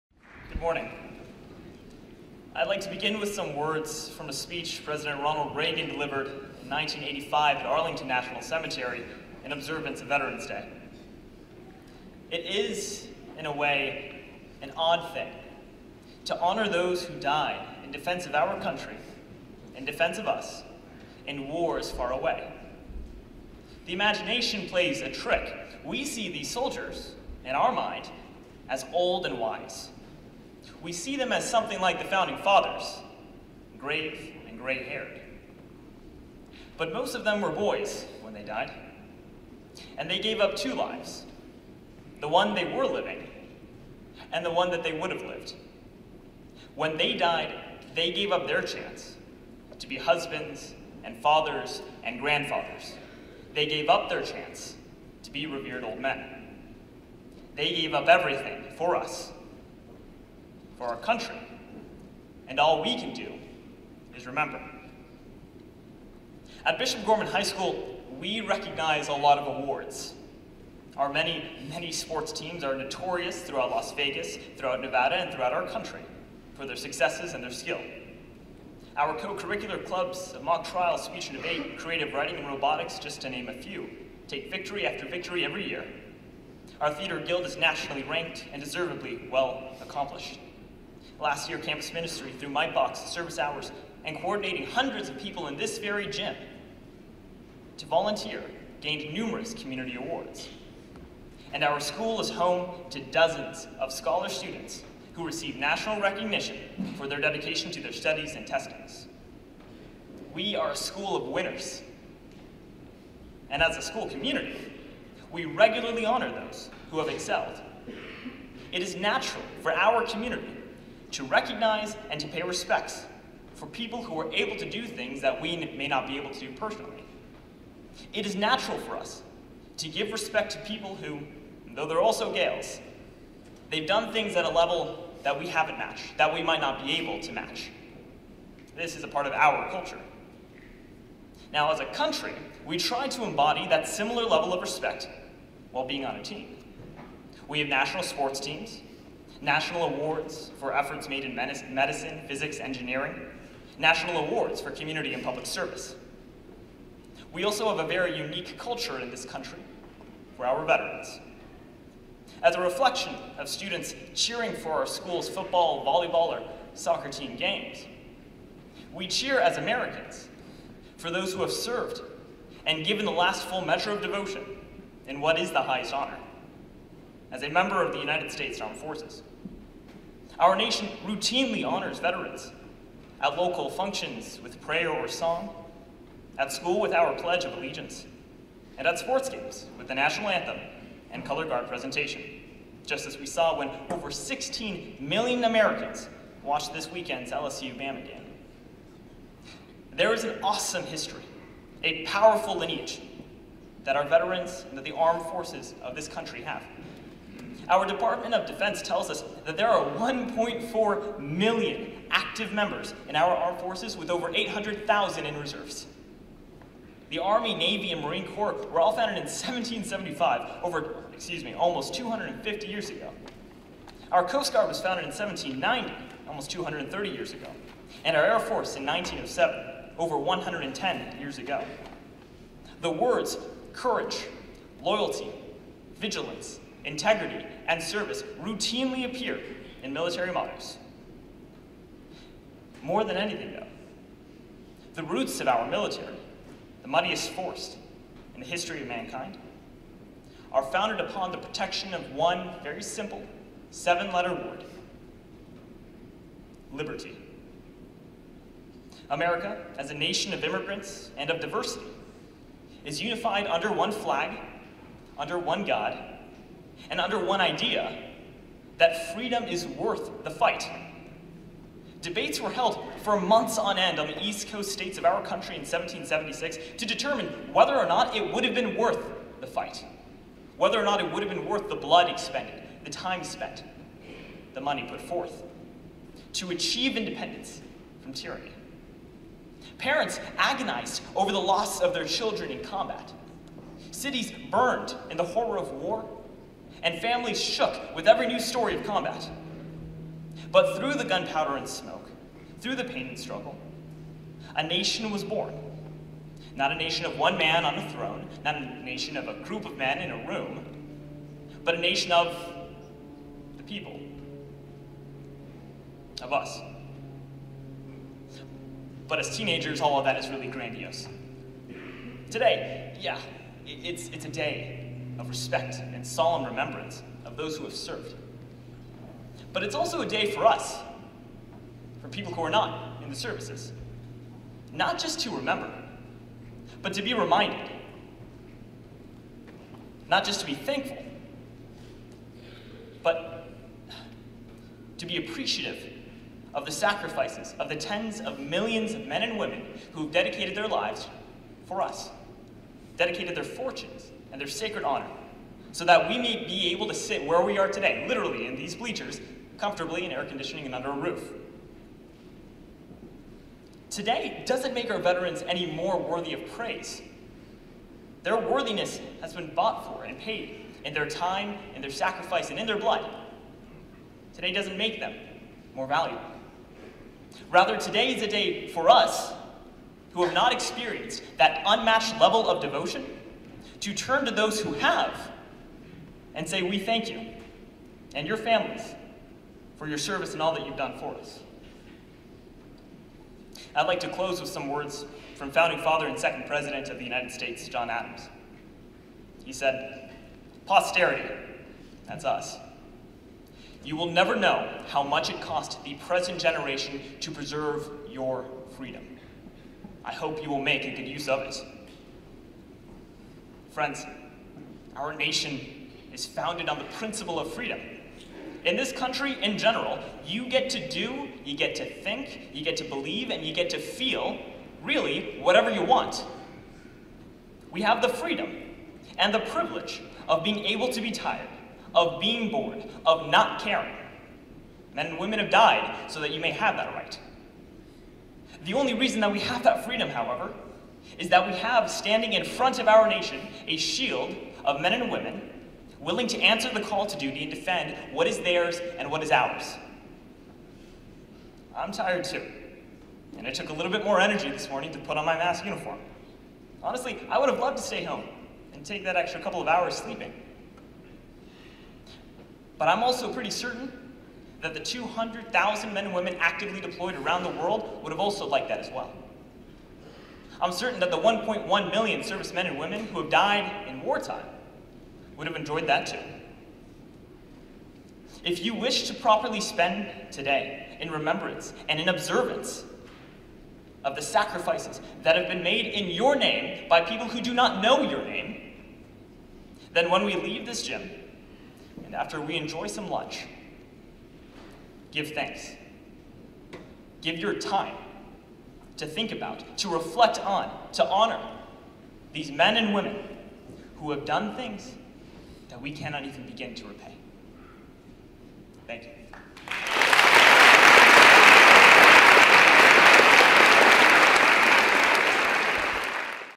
BGHS 2019 Veterans Day Speech
An original speech I had written and delivered on November 11th, 2019 at the BGHS Annual Veterans Day Celebration.